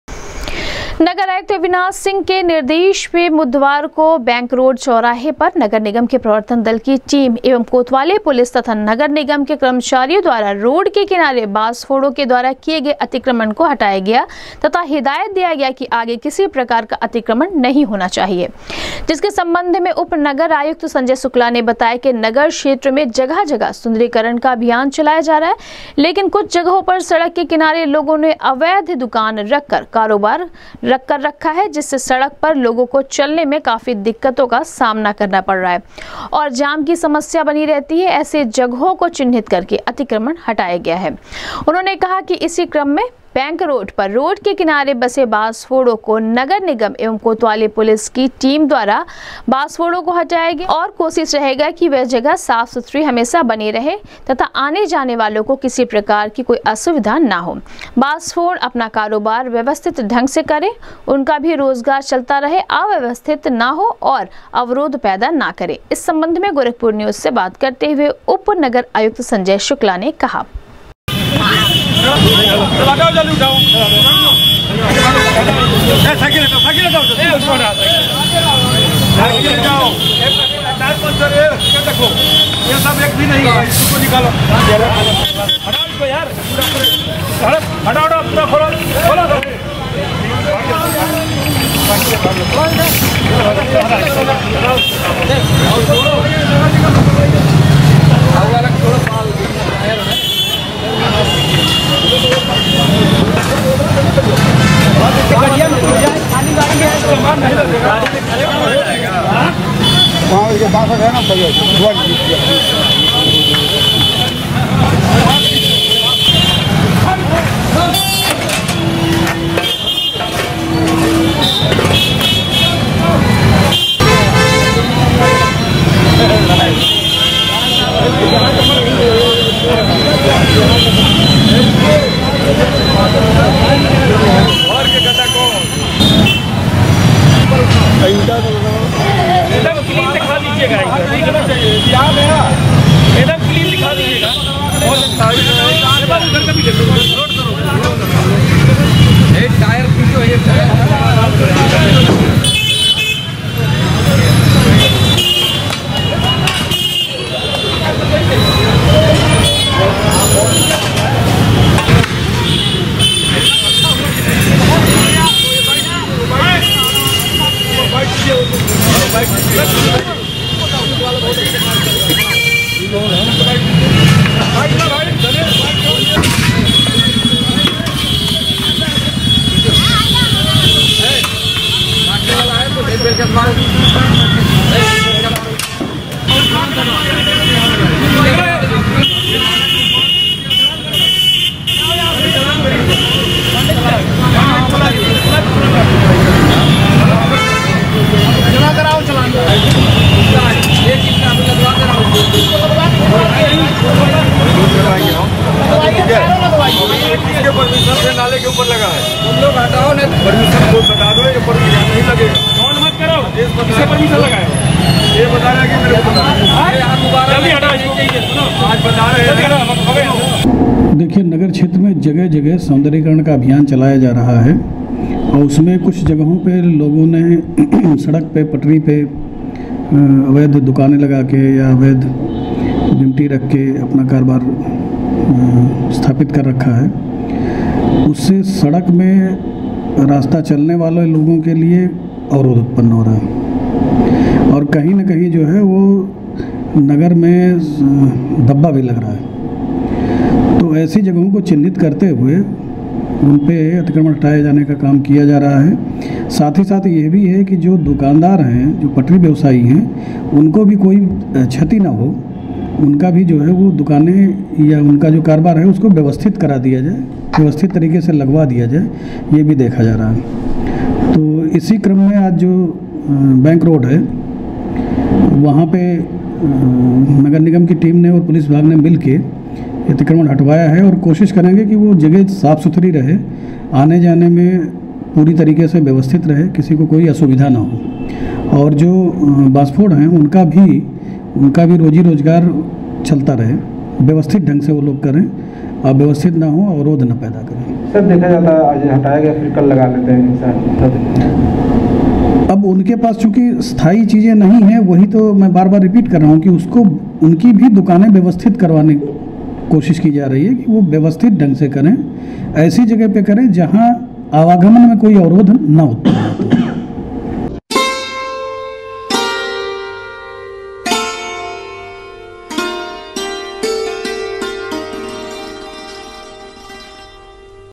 बैंक रोड चौराहे पर रोड के किनारे बांसफोड़ो के विरुद्व अभियान चलाकर नगर निगम के प्रवर्तन दल की टीम एवं
बैंक रोड चौराहे पर रोड के किनारे बांसफोड़ो के विरुद्व अभियान चलाकर नगर निगम के प्रवर्तन दल की टीम एवं कोतवाली पुलिस तथा नगर निगम के कर्मचारियों ने हटवाया अतिक्रमण, उप नगर आयुक्त संजय शुक्ला ने कहा, दी गई है हिदायत, आगे किसी प्रकार का नहीं होना चाहिए अतिक्रमण